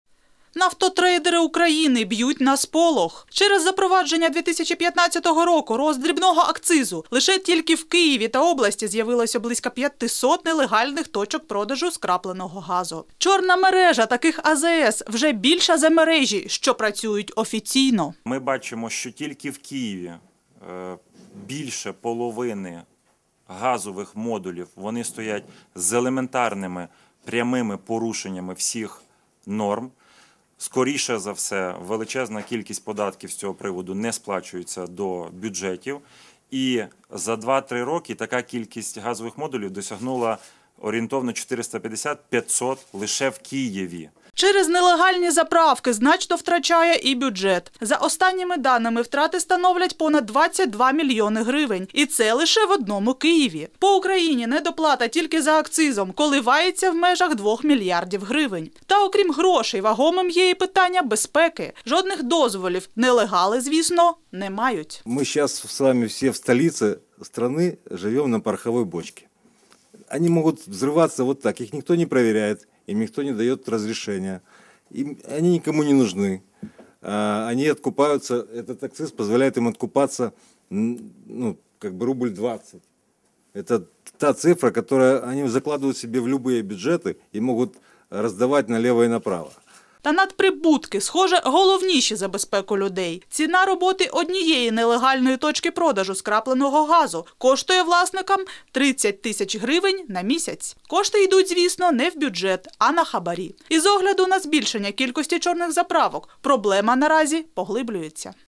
Половина сжиженного газа в Киеве продается нелегально
Только в Киеве и области появилось около 500 нелегальных точек продажи сжиженного газа.